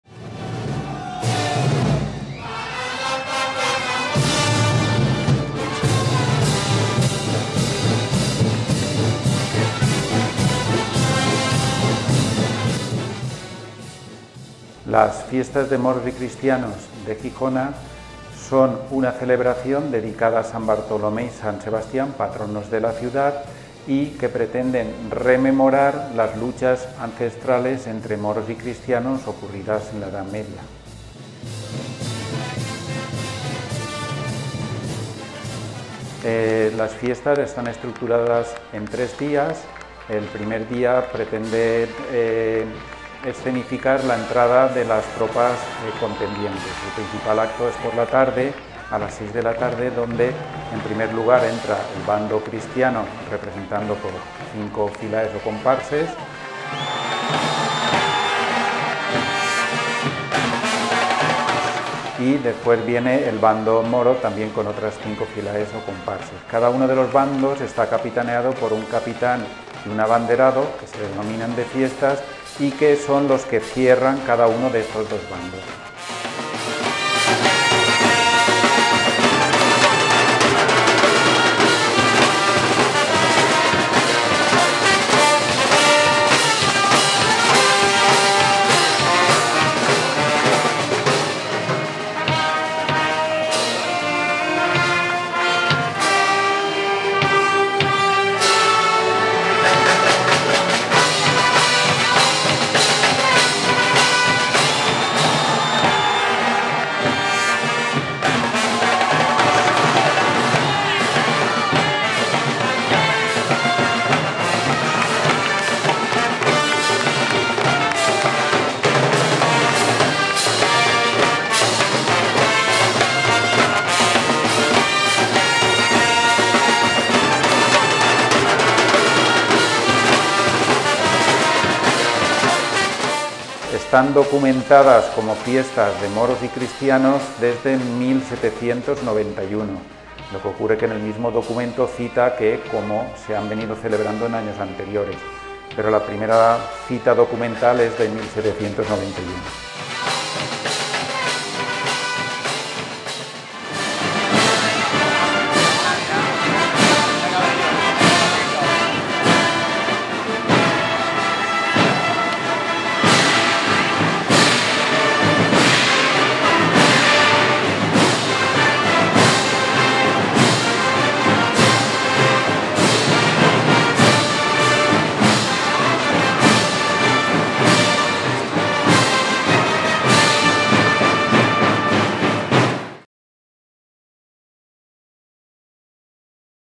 Fiestas de Moros y Cristianos de Jijona
• Usos sociales, rituales y actos festivos

La Villa de Jijona (Alicante) celebra cada año sus fiestas de Moros y Cristianos que conmemoran la lucha ancestral que hubo entre estos dos bandos en la Edad Media. L@s jijonenc@s se visten con increíbles trajes y desfilan en sus “filaes” capitaneados por un capitán y un abanderado. Estas fiestas llenas de ilusión y espectáculo tienen origen en la Edad Moderna sobre todo en los siglos XVI y XVII.